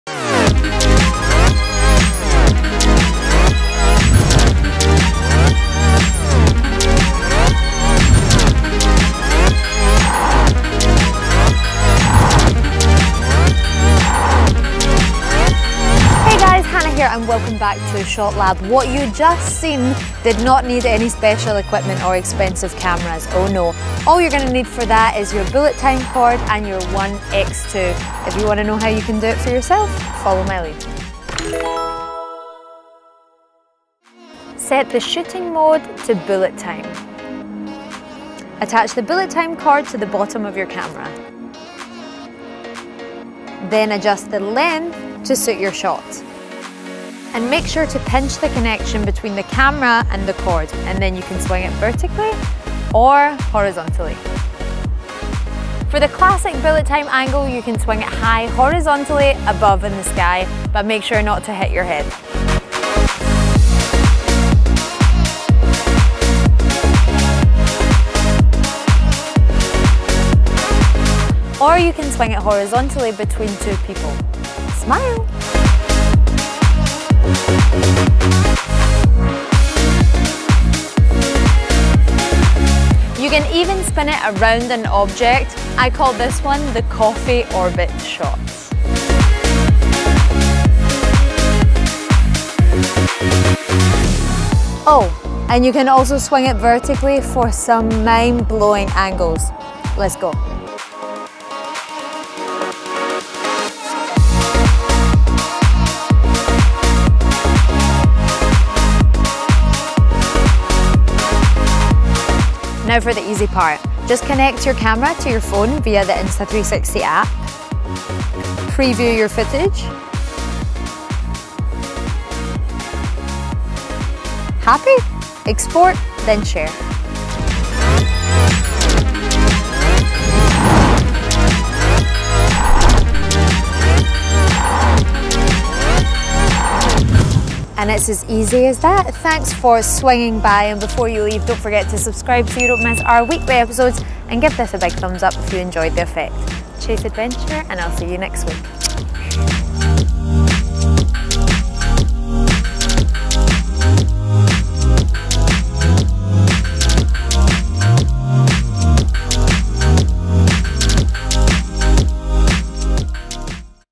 Bullet Time Cord - Become the Slow Motion MASTER!!
The Bullet Time Cord is a simple accessory with a ton of cool ways to use it! In this episode we show you some of the shots you can get using the Bullet Time Cord, how to shoot them, and how to edit using Shot Lab in the Insta360 app. We used ONE X2 for the video but this accessory is compatible with ONE R, ONE X and ONE. Yet another way you can get fun, creative shots with your Insta360 camera!